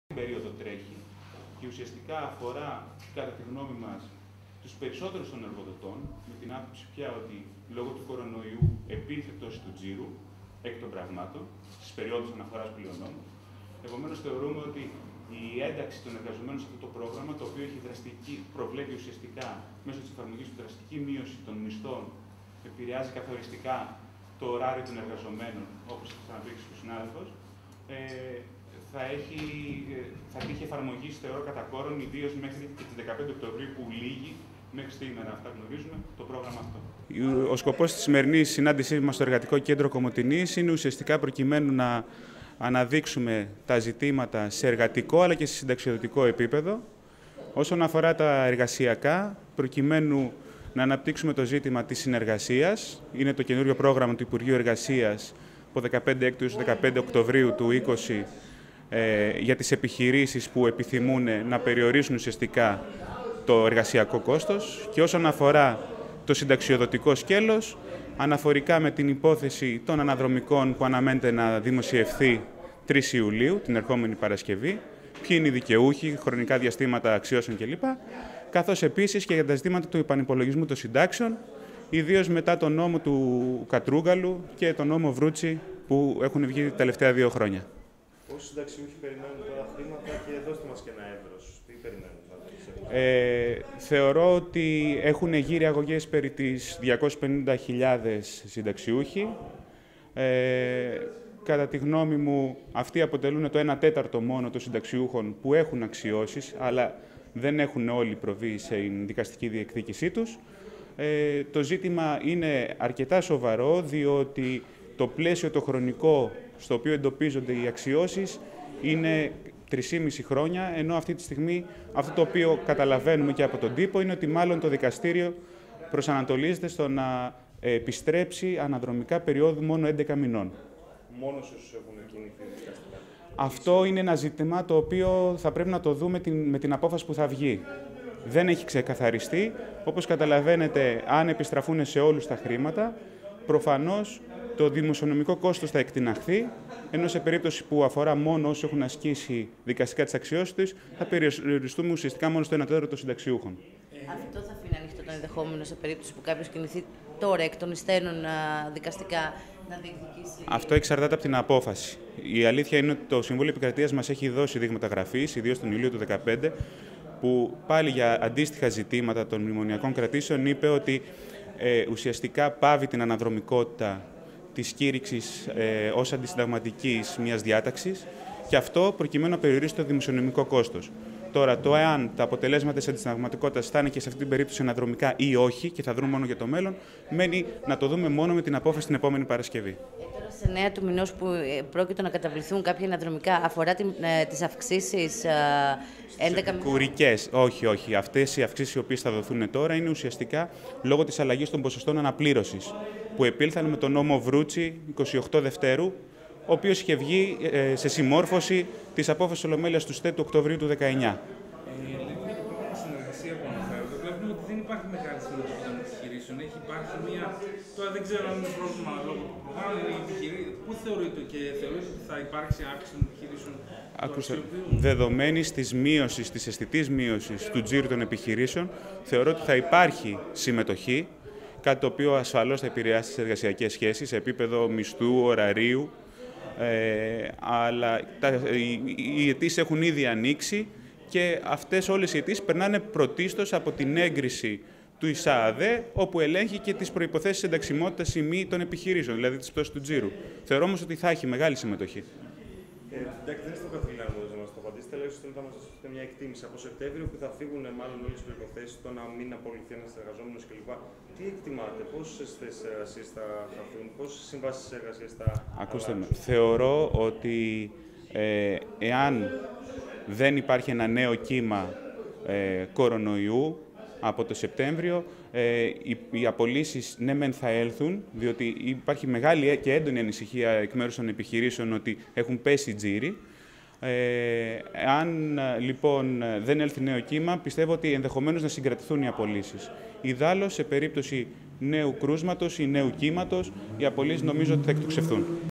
thrakionline.gr: Αναδρομικά, αυξήσεις επικουρικών και νέα μισθολόγια στο επίκεντρο ημερίδας
thrakionline.gr: Ενημέρωση για συνταξιοδοτικά και μισθολογικά θέματα πραγματοποίησε το πρωί της Παρασκευής στο εργατικό κέντρο ο δικηγόρος εργατολόγος από την Αθήνα Χρήστος Βουρλιώτης. Ας δούμε στις δηλώσεις του όσα μας είπε για αναδρομικά, αυξήσεις επικουρικών και για το πρόγραμμα συνεργασία.